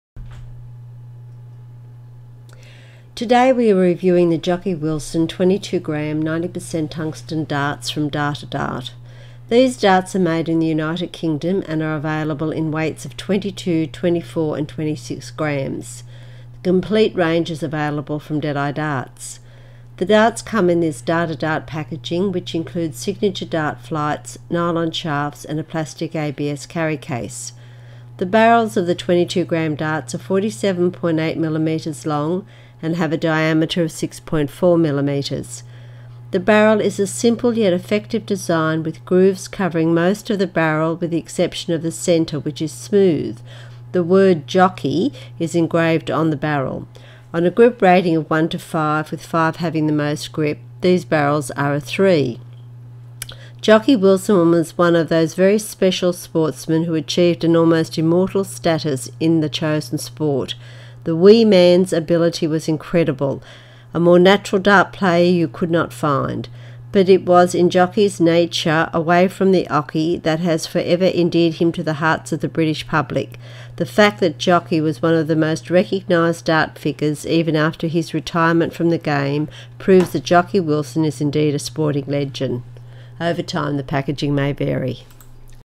Jocky Wilson 22 gram 90% Tungsten Darts
Today we are reviewing the Jocky Wilson 22 gram 90% Tungsten Darts from Datadart - these darts are made in the United Kingdom and are available in weights of 22, 24 and 26 gram.  The complete range is available from Deadeye Darts.

The darts come in this Datadart packaging which includes signature dart flights, nylon shafts and a plastic ABS Carry Case.

The barrels of the 22 gram darts are 47.8mm long and have a diameter of 6.4mm

The barrel is a simply yet effective design with grooves covering most of the barrel with the exception of the centre which is smooth. The word "JOCKY" is engraved on the barrel.  On a grip rating of one to five with five having the most grip, these barrels are a three.

Jocky Wilson was one of those very special sportsmen who achieve an almost immortal status in their chosen sport. The "Wee Man's" ability was incredible, a more natural dart player you could not find. But it was in Jocky's nature away from the oche that has forever endeared him to the hearts of the British public. The fact that Jocky was one of the most recognized dart figures even after his retirement from the game proves that Jocky Wilson is indeed a sporting Legend.

Over time the packaging may vary.